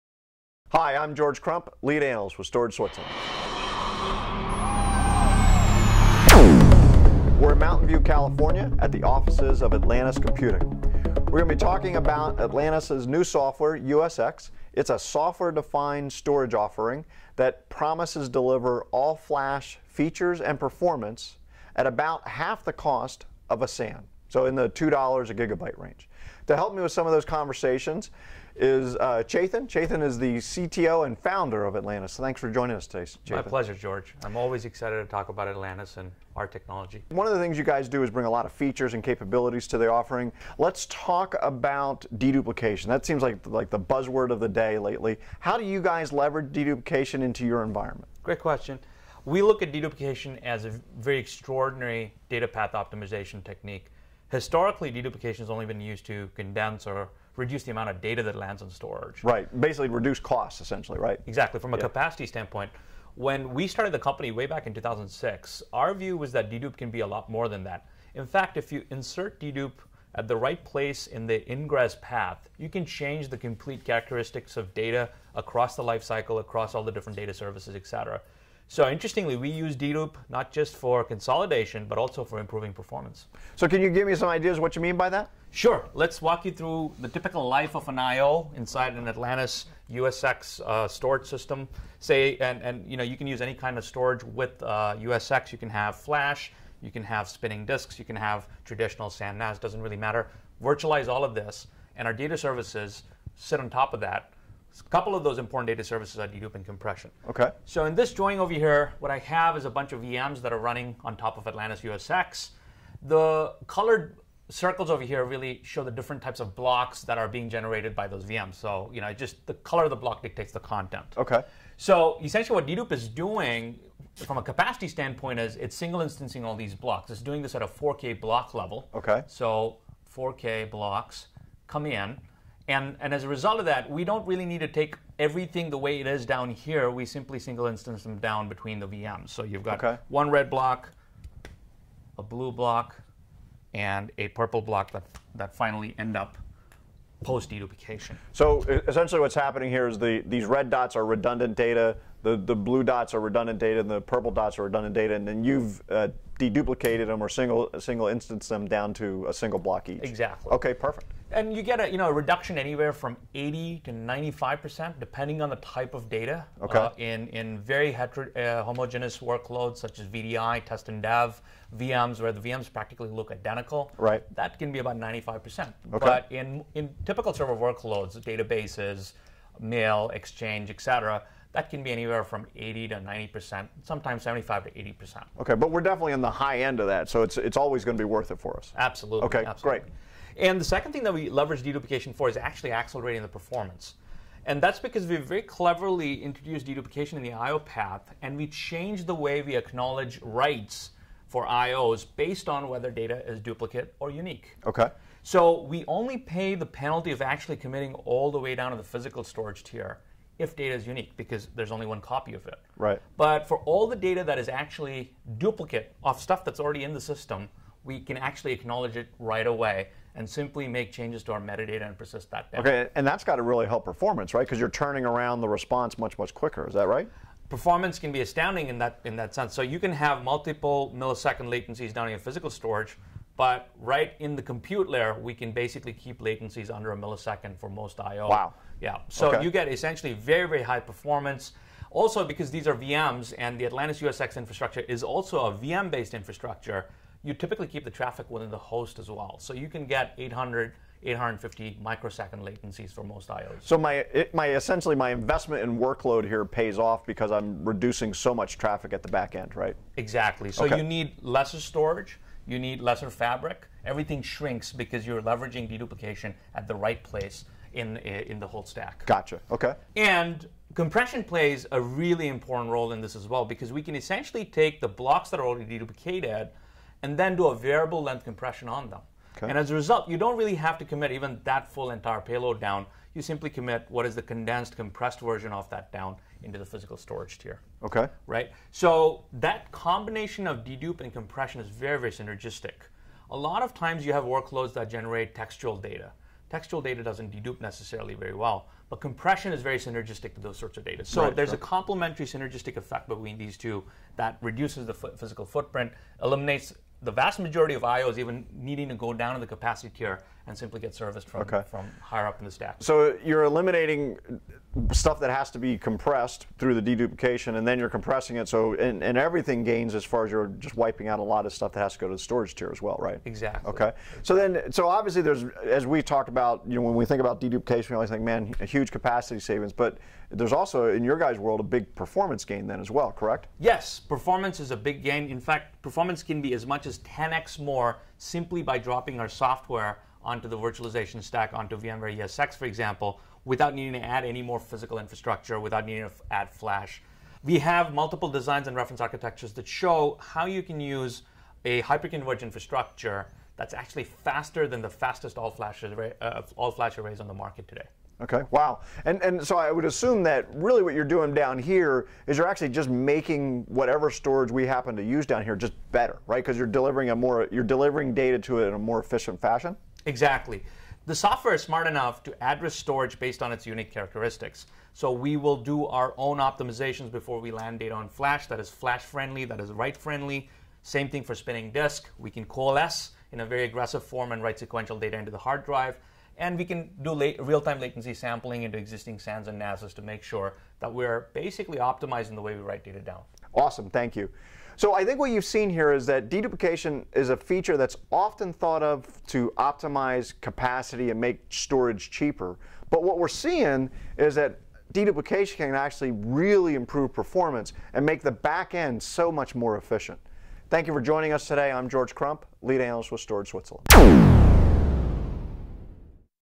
3692 1 Technology Data Reduction
Chetan Venkatesh reviewing the Data Reduction features of USX with Storage Switzerland